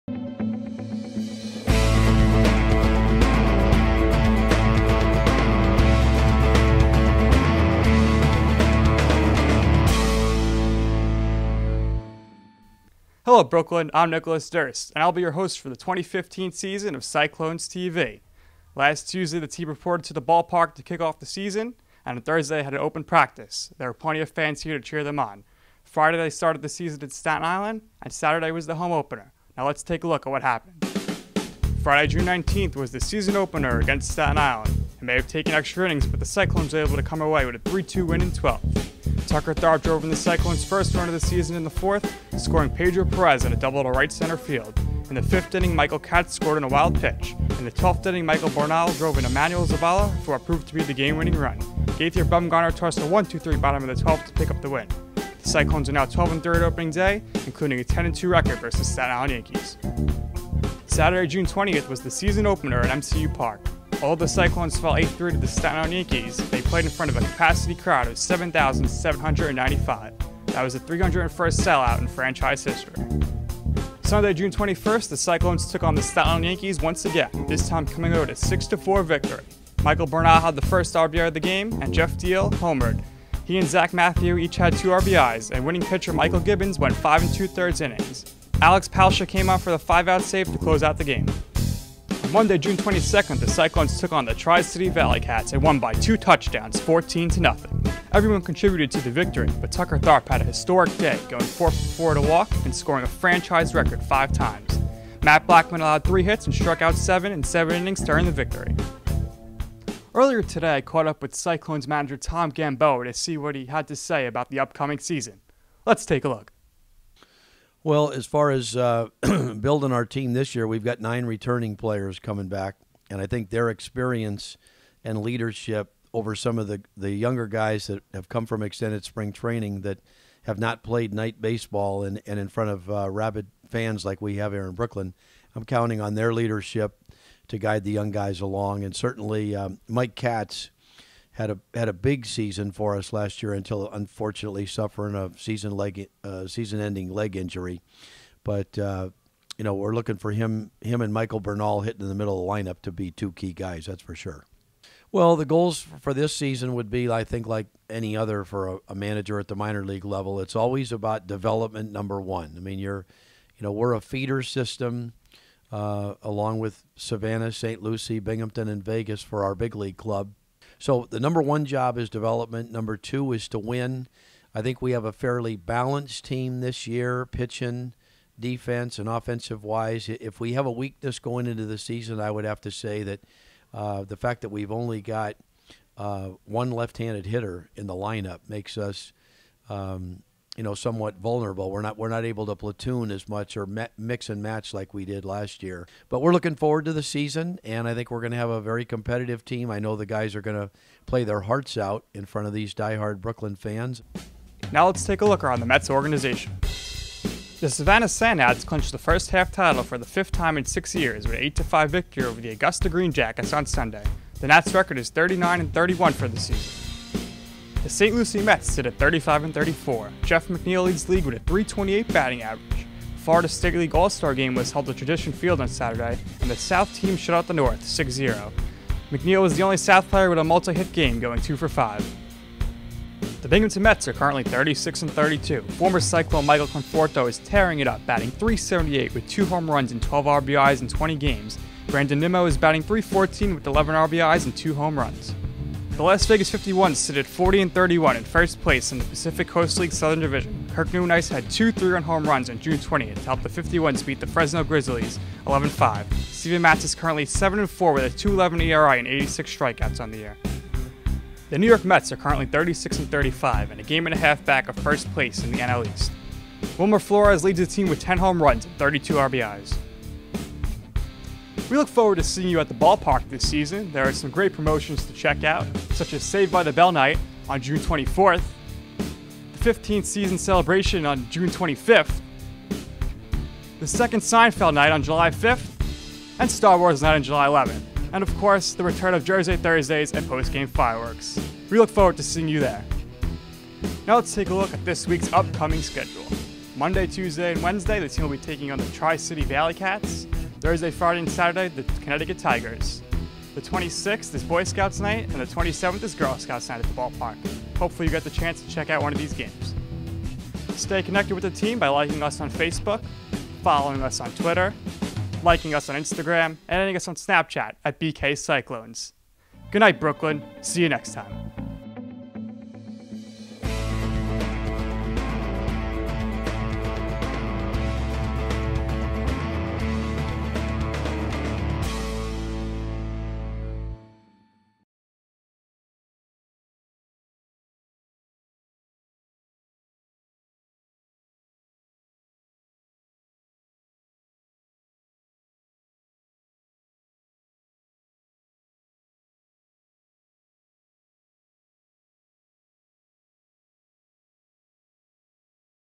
Cyclones TV Week One
A look back at the first week of Brooklyn Cyclones Baseball.